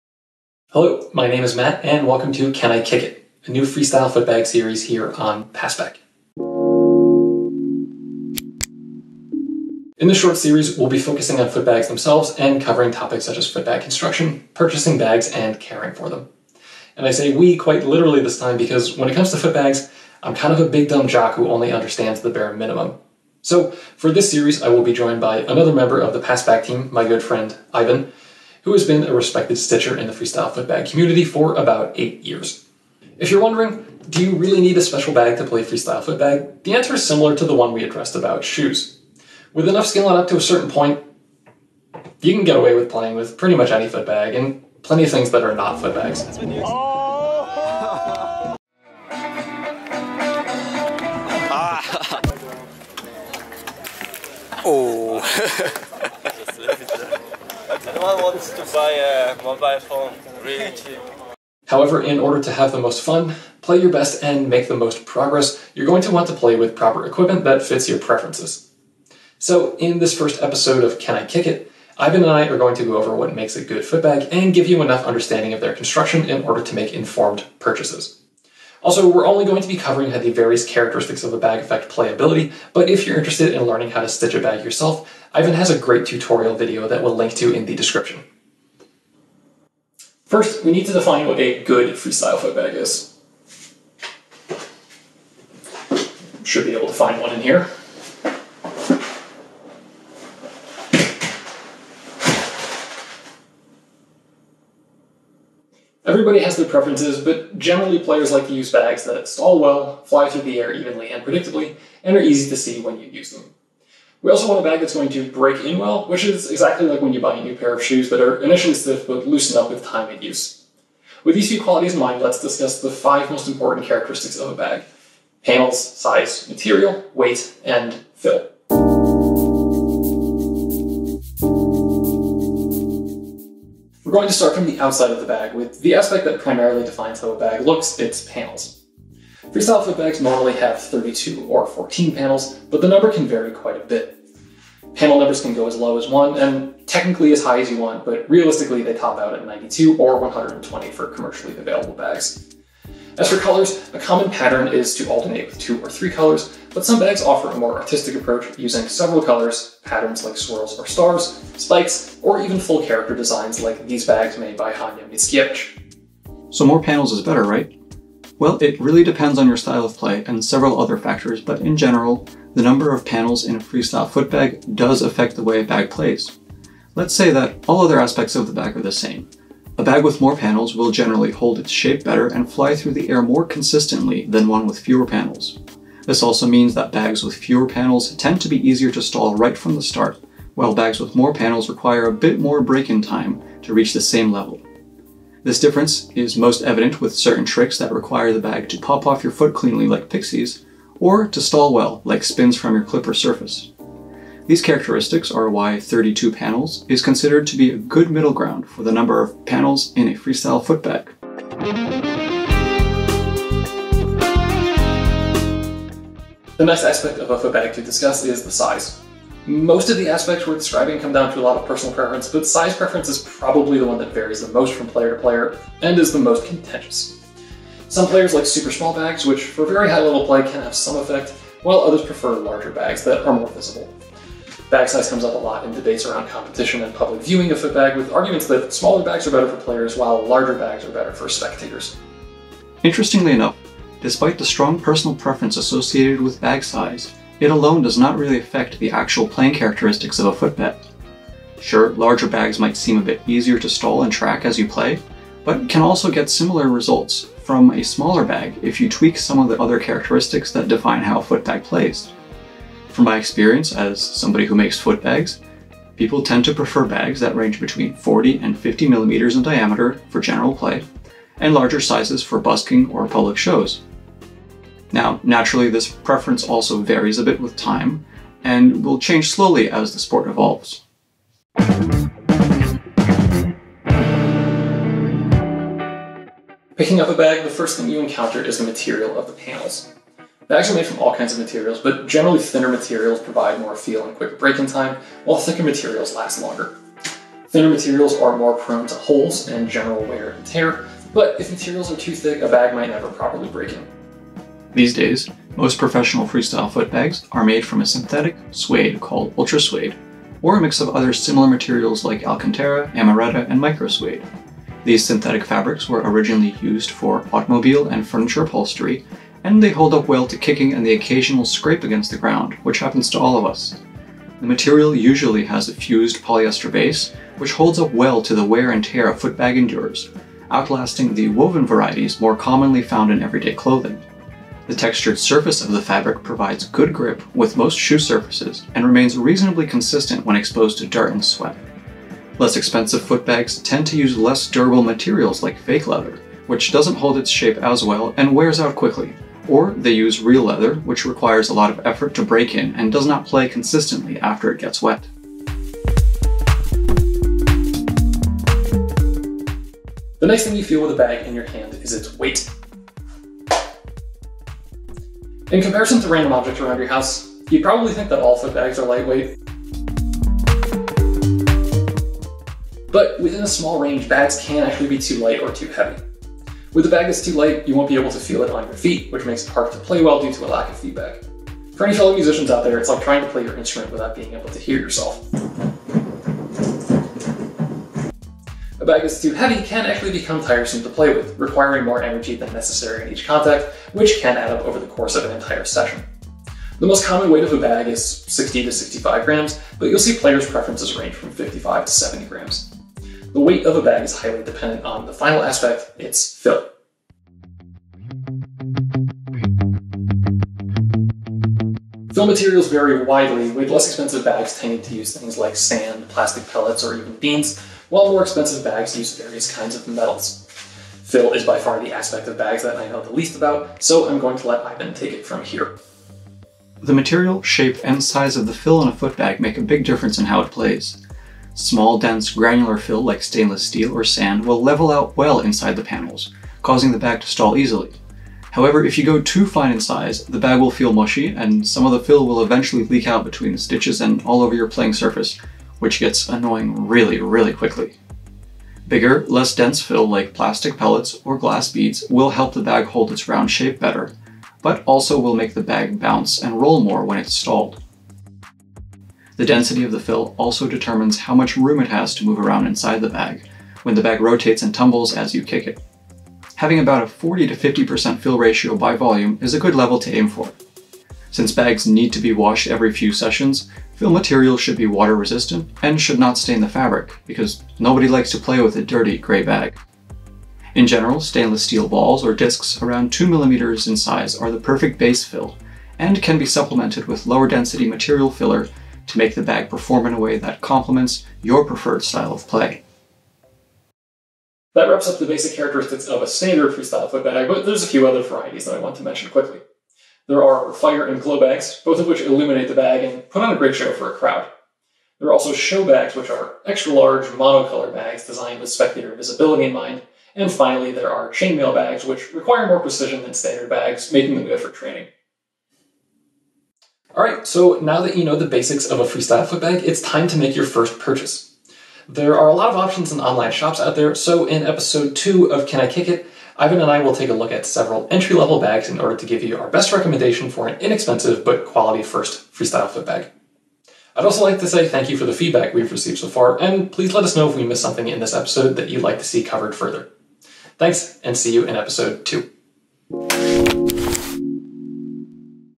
Can I Kick It? | Ep. 1: What's in a Bag?
Introducing "Can I Kick It?", a series all about freestyle footbags (hacky sacks). In Episode 1, Matt and Ivan break down the ins and outs of freestyle footbags and cover the most important characteristics that affect bag playability. Come back for Episode 2 where we will review a collection of entry-level freestyle footbags.

0:00 - Intro
1:30 - What is a Good Bag?
2:09 - Panels
3:32 - Size
4:41 - Fabric
5:56 - Weight
6:47 - Filler
8:14 - Non-Standard Bags
8:37 - Outro